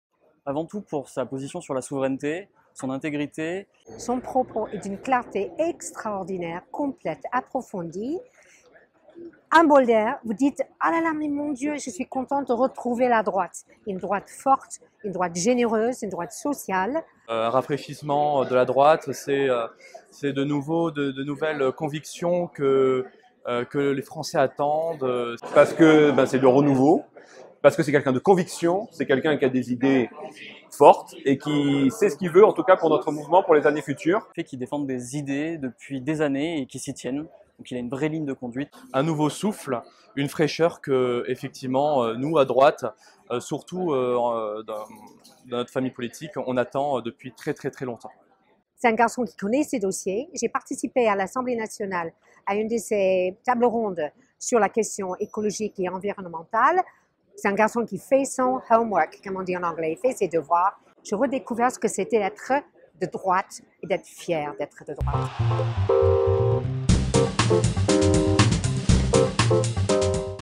🗣 Paroles de militants #4
🗣Vous avez été nombreux à prendre la parole à l’occasion de mes déplacements 👇